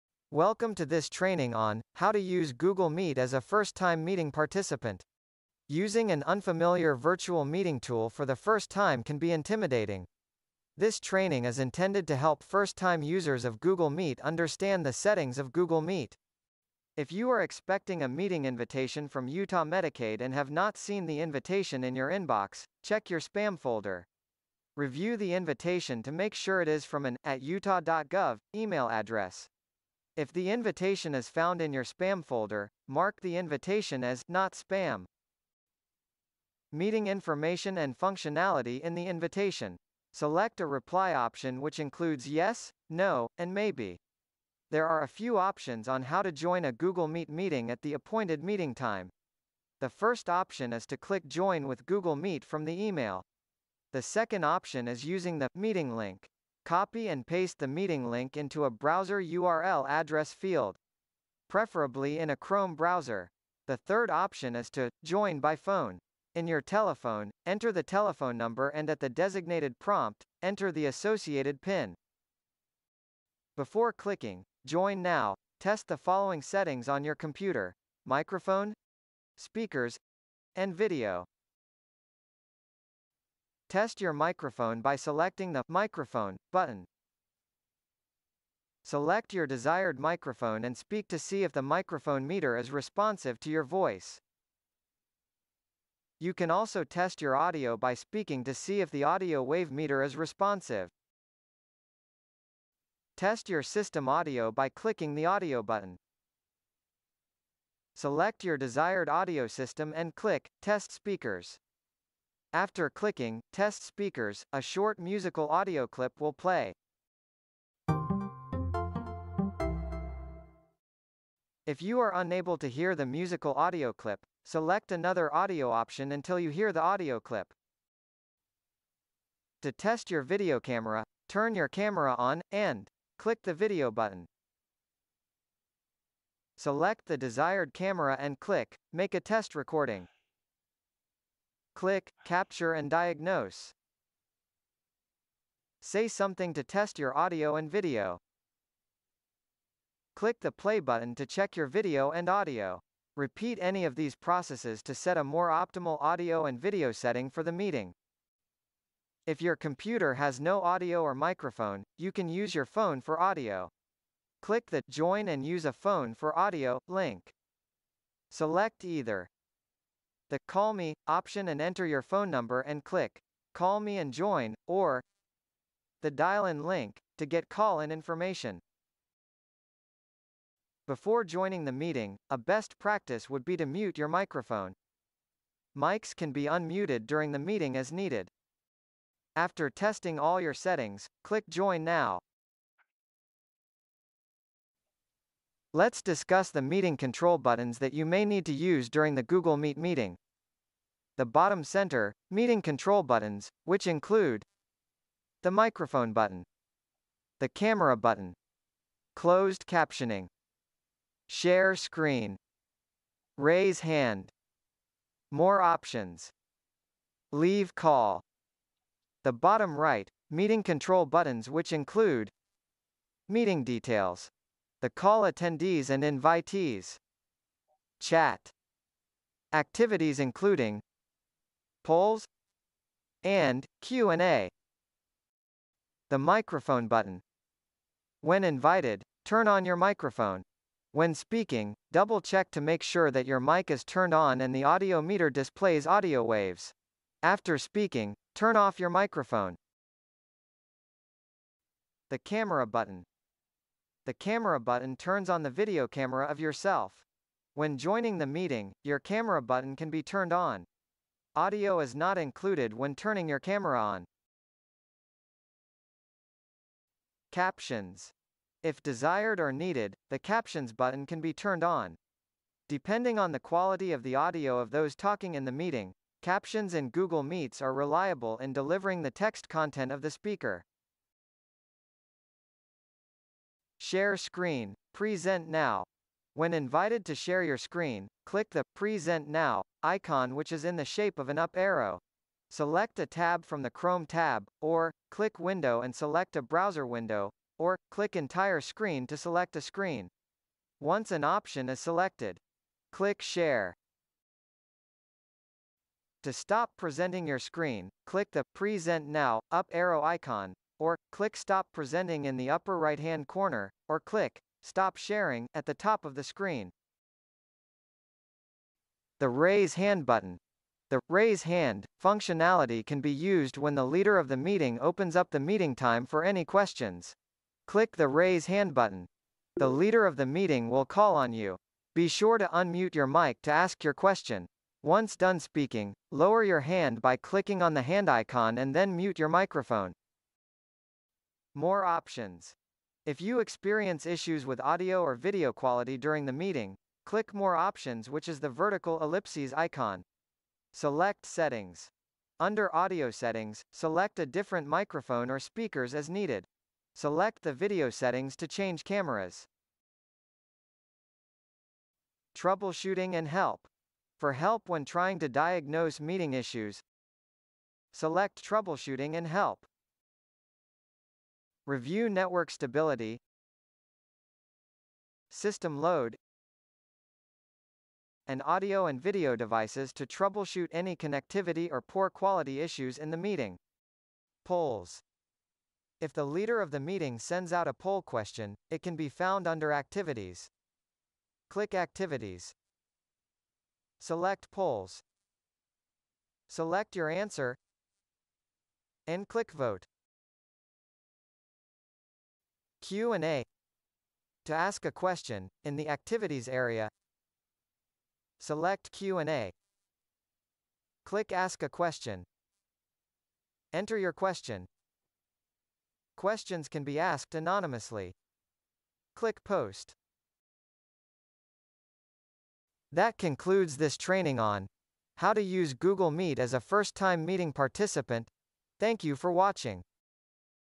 How To Use Google Meet As a First Time Meeting Participant
Using an unfamiliar virtual meeting tool for the first time can be intimidating. This training is intended to help first-time users of Google Meet understand the settings of Google Meet.